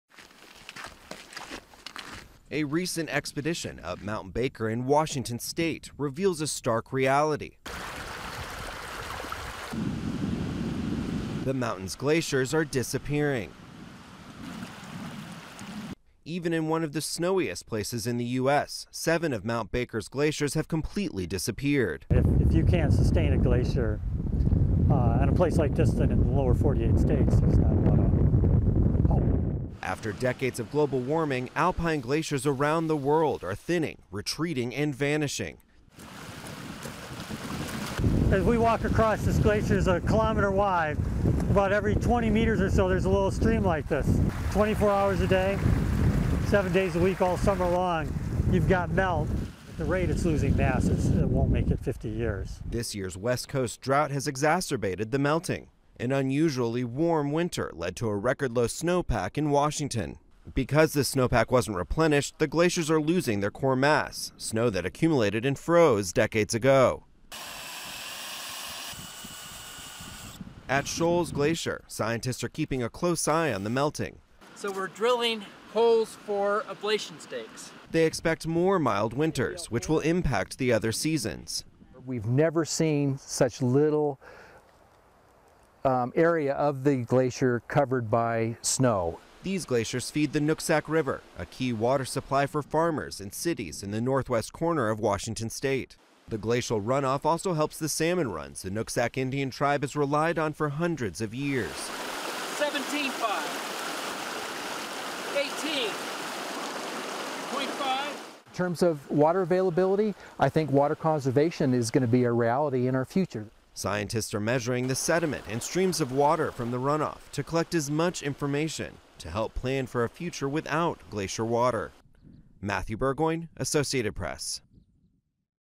Climate Change Shrinking Mountain WA Glaciers
Even in one of the snowiest places in the continental United States, glaciers are disappearing. Scientists are studying glaciers in Washington state’s Mount Baker are preparing for a future without them. (Aug. 27)